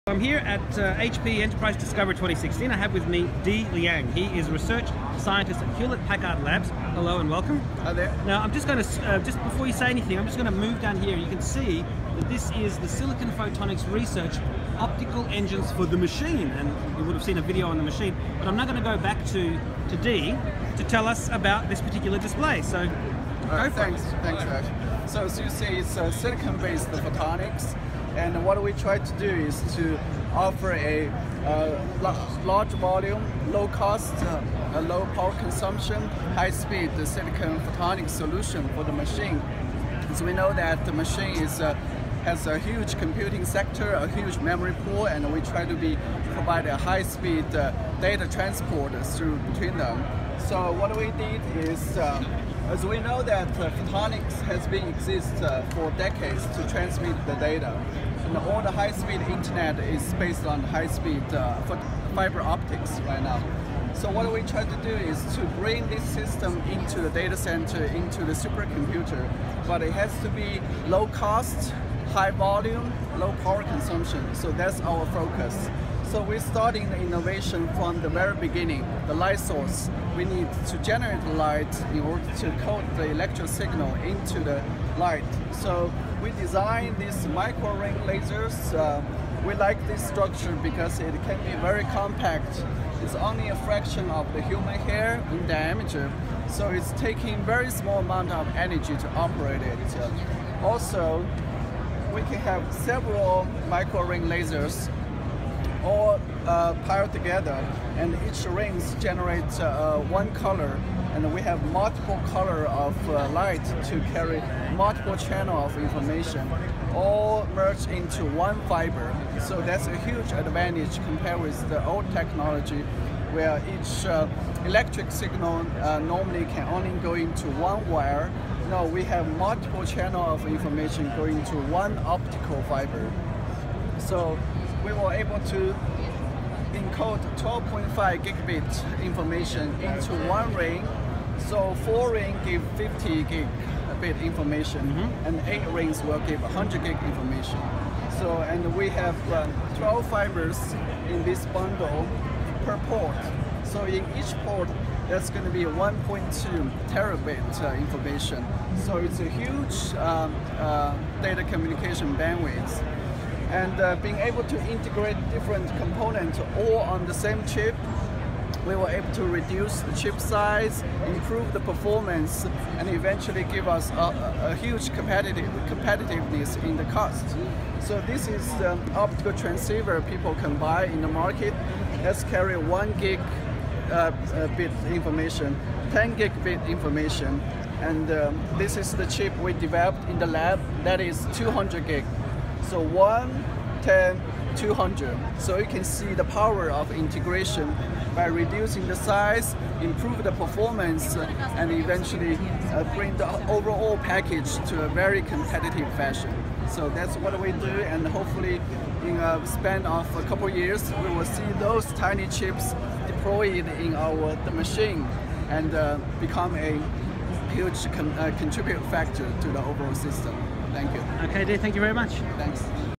HPE research scientist Di Liang explains Silicon Photonics Research with 'The Machine'
HPE research scientist Di Liang explains Silicon Photonics Research with 'The Machine' at HPE Discover 2016

One in a series of videos looking at The Machine stand at Discover 2016 - See the rest of the videos here: